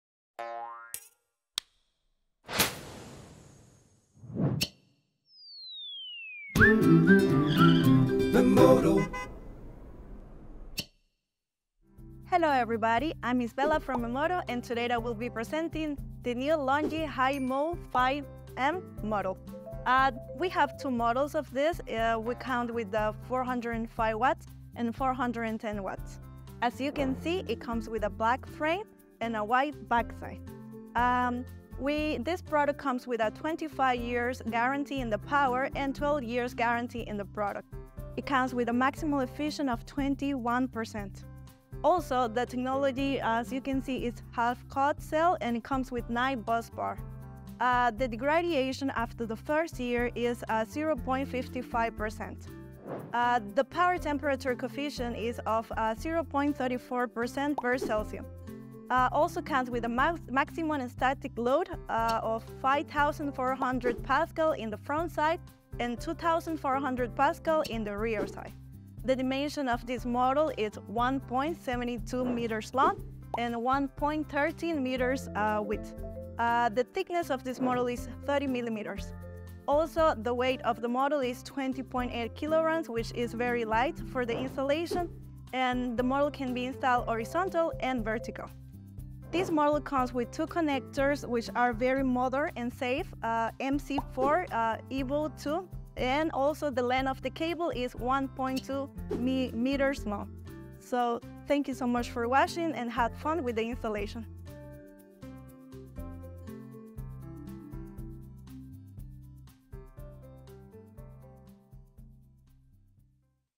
Checked out in 2 Mins: Longi Hi-MO 5m
Energy:Expert Isabela introduces you to the Longi Hi-MO 5m , its features and what sets it apart. 💡

Fact check:

• 410 W
• Black module frame
• White back foil
• Snow load: 5400 Pa
• Wind load: 2400 Pa
• Dimensions: 1722 x 1134 x 30 mm
• Weight: 20.8 kg

• 12 years product warranty
• 25 years performance warranty

Technical updates and changes may apply. All information without guarantee.